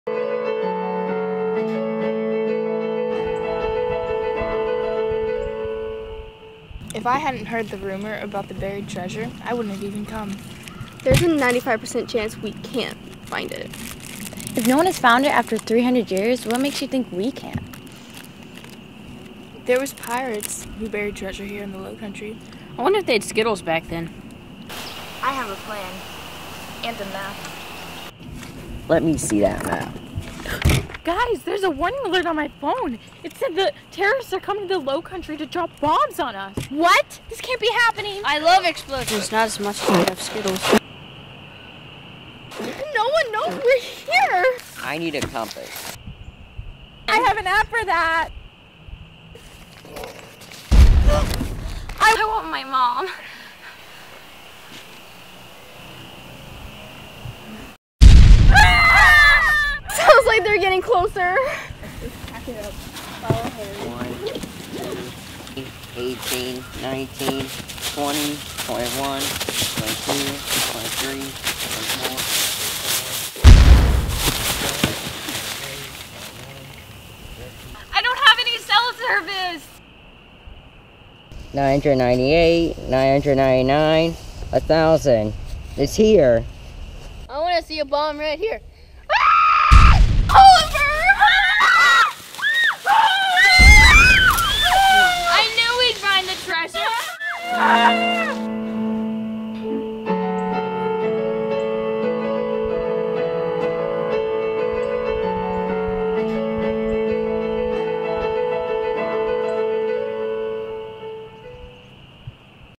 Apocalyptic Park
Teens go to a park to dig up 300 year old pirate treasure when a surprise explodes upon them.